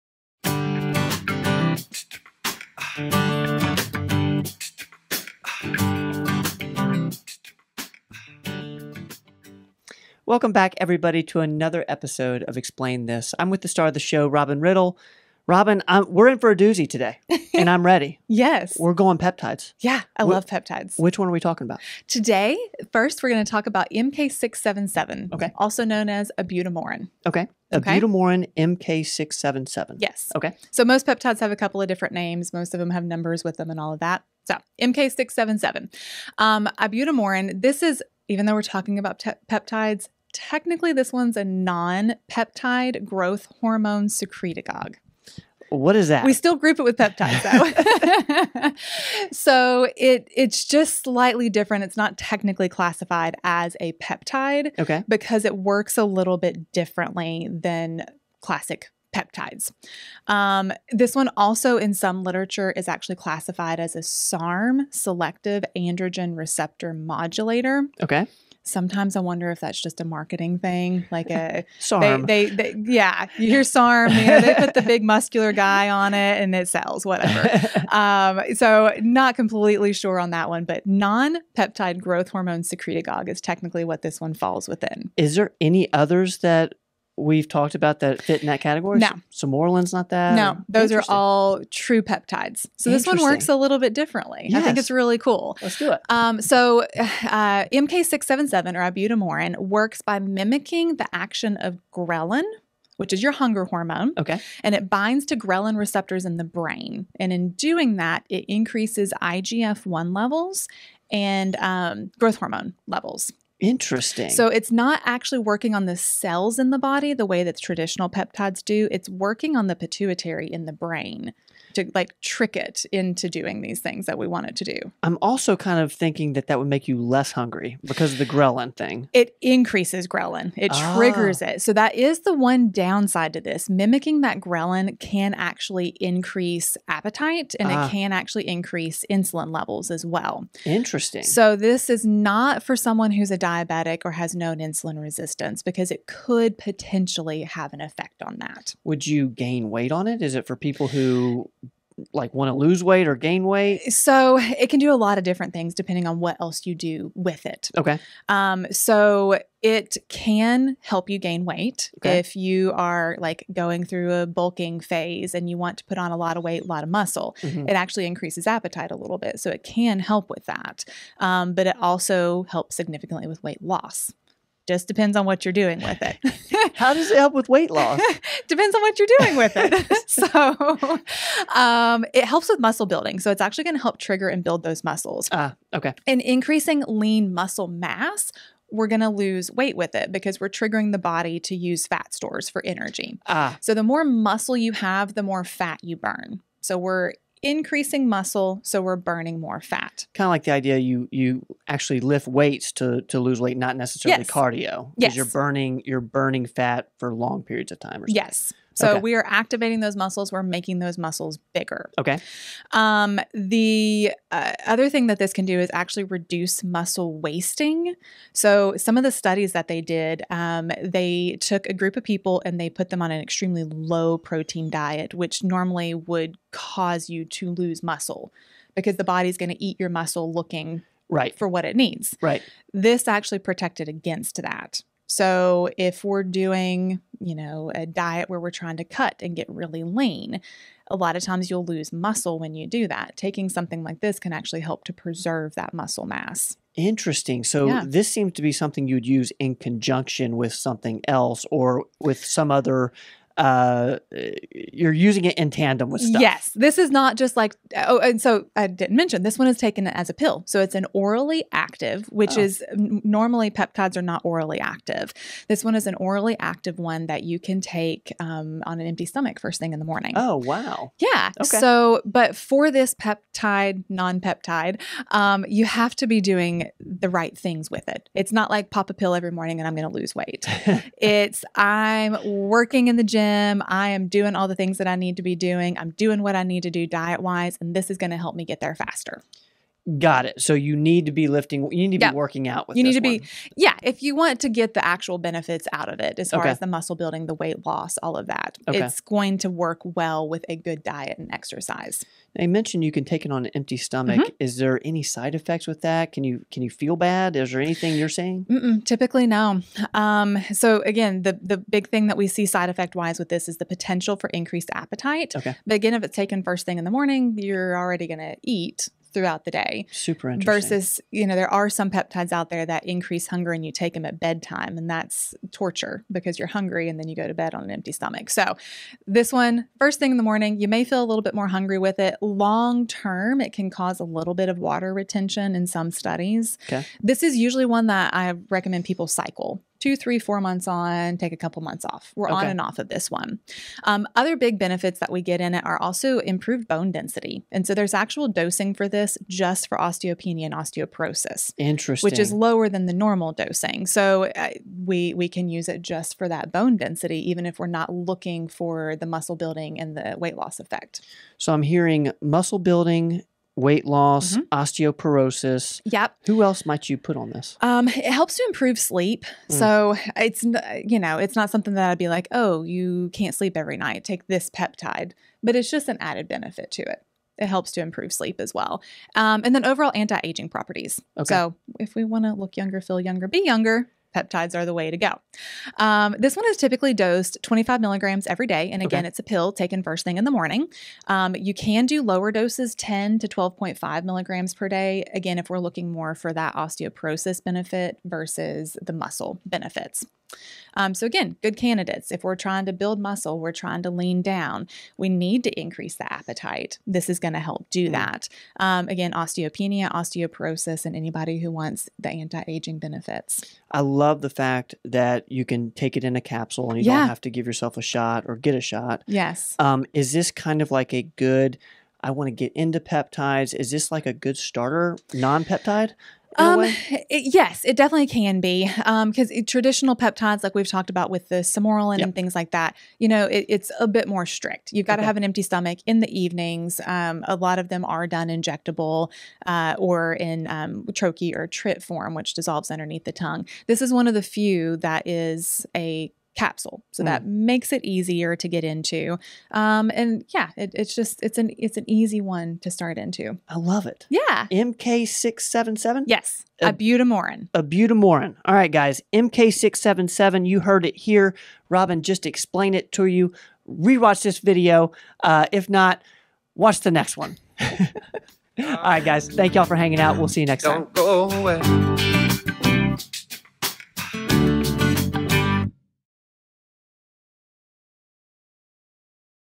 MK-677 IBUTAMOREN I Explain This with Robin Riddle, FNP-C
In this episode, Robin Riddle, FNP-C explains what MK-677 (Ibutamoren) is, how it works, what it does for you, a few side effects, how to dose, and who is a good candidate!  If you are interested in healthy aging and/or building muscle, this video is for you!